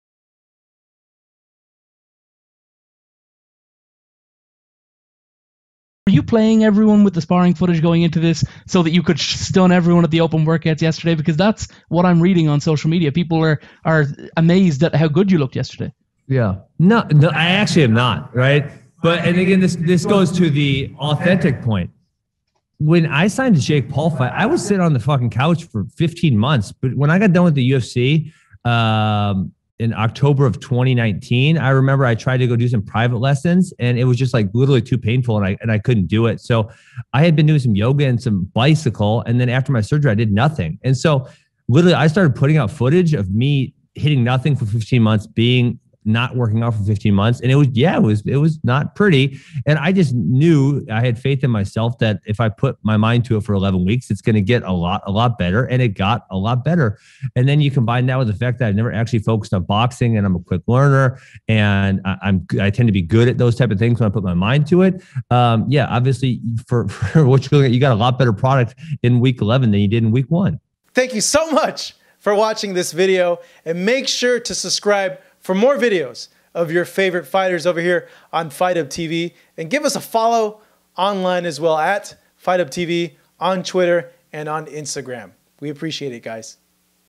BEN ASKREN OPENS UP ON IF HE WAS TROLLING EVERYONE WITH TERRIBLE STRIKING & SPARRING FOOTAGE
Ben Askren finally opens up on if he was trolling the world with the sparring and striking footage that was released of him when he first started to train for his Jake Paul fight as Ben says he wasnt trolling but that was video and footage of him without any sort of traininig for 16 months, so naturally he was gonna look bad.